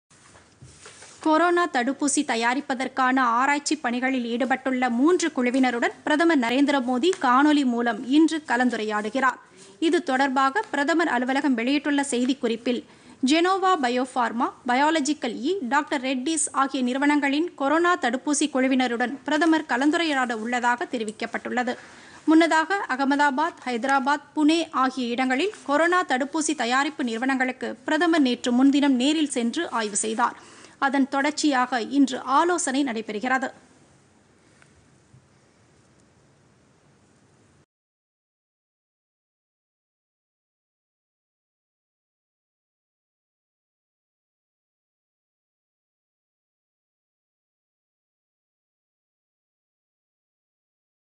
தடுப்பூசி தயாரிப்புக்கு ரூ. 900 கோடி
கொரோனா தடுப்பூசி தயாரிப்புப் பணிகளுக்கு தொள்ளாயிரம் கோடி ரூபாய் ஒதுக்கீடு செய்தது மத்திய அரசு - தடுப்பூசி தயாரிப்புக்குழுக்களுடன்  பிரதமர் நரேந்திர மோதி இன்று ஆலோசனை